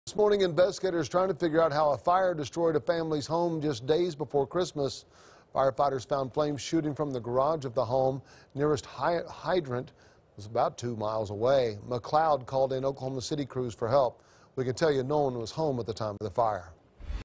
Home destroyed after fire in McLoud
A home was destroyed in a fire